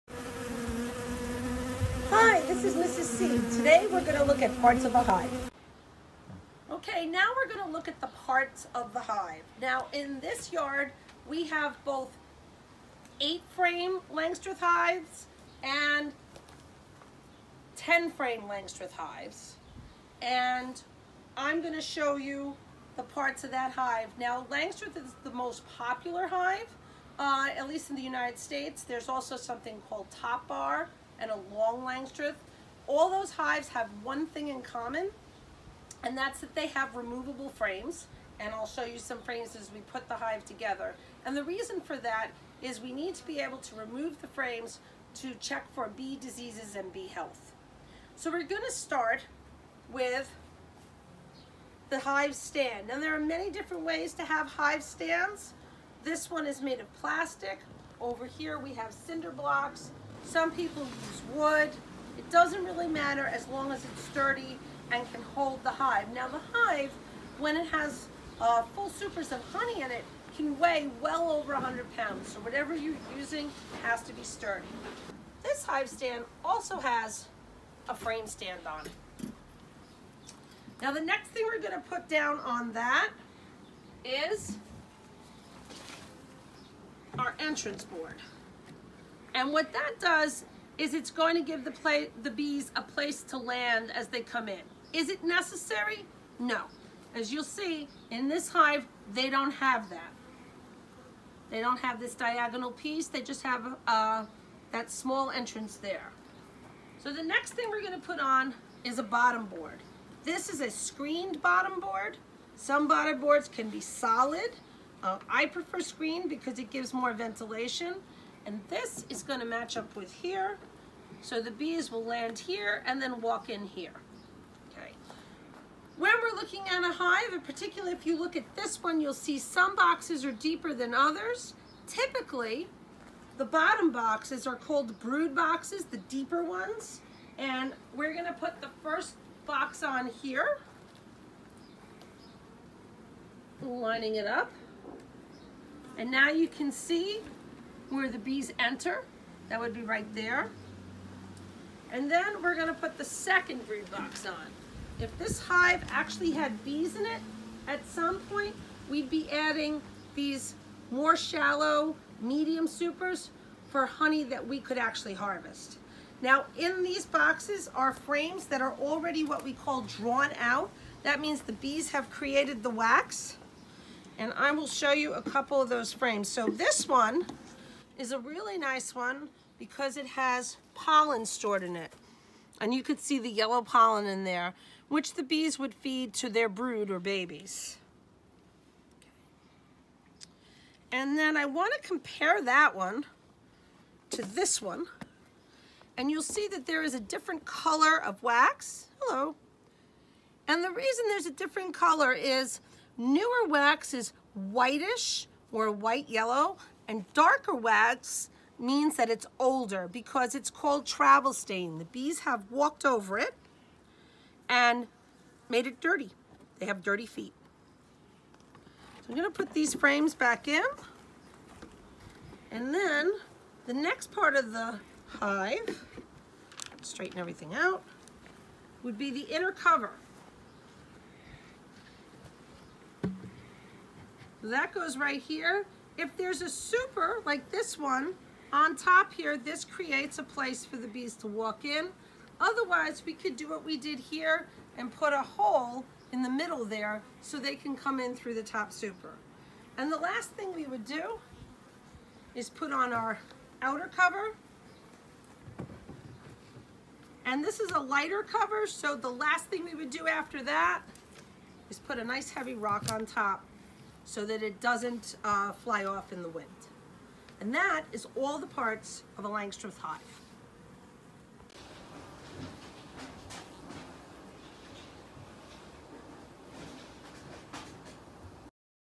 Parts of the Beehive with Mrs. C
Let's look inside a Langstroth beehive with Mrs. C!